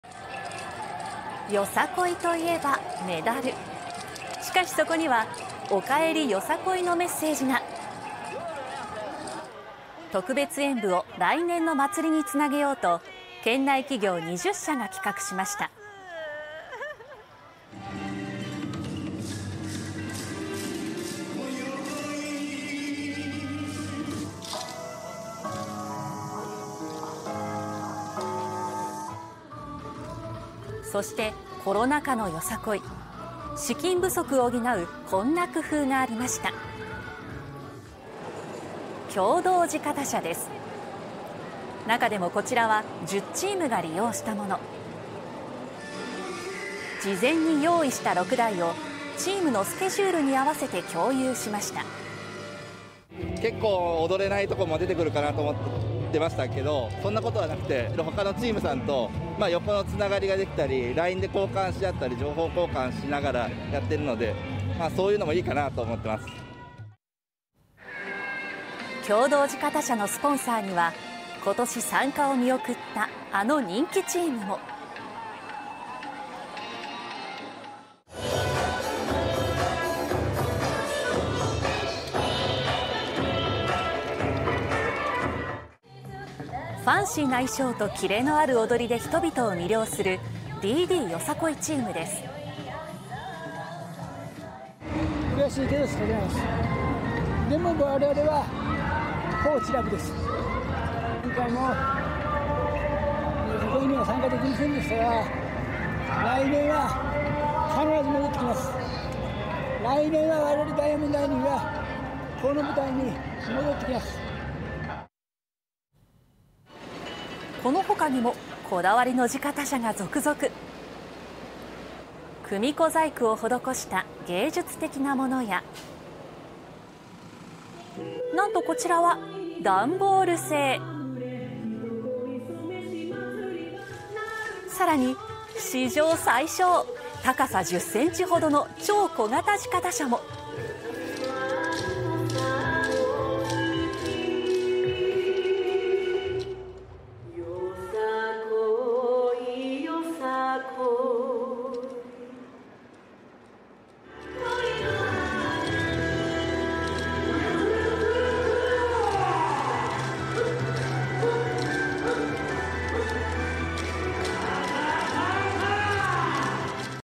“おかえり”メダルに共同地方、3年ぶり『よさこい』様々な工夫が【高知】 (22/08/15 16:00)
「よさこい」と言えばメダル。しかしそこには「おかえりよさこい」のメッセージが。特別演舞を来年の祭りにつなげようと県内企業20社が企画しました。

そしてコロナ禍のよさこいには、こんな工夫がありました。「共同地方車」です。資金不足などを理由に自前で地方車を構えることができなかった10チームに6台が貸し出されました。

利用チーム:
結構踊れないとこも出てくるかなと思ってましたけど、他のチームさんと横のつながりができたり、LINEで情報交換しながらやってるので、そういうのもいいかなと思ってます

共同地方車のスポンサーには今年参加を見送ったあの人気チームの名前も。ファンシーな衣装とキレのある踊りで人々を魅了する「DDよさこいチーム」です。

DDホールディングス・松村厚久 社長:
（チームが参加できず）悔しいけど仕方ないです。でも我々は”高知LOVE!”です。今回もよさこいには参加できませんでしたが、来年は必ず戻ってきます。来年は我々DDがこの舞台に戻ってきます

この他にも、こだわりの地方車が続々。組子細工を施した芸術的なものや、ダンボール製、さらに史上最小・高さ10センチほどの超小型地方車も。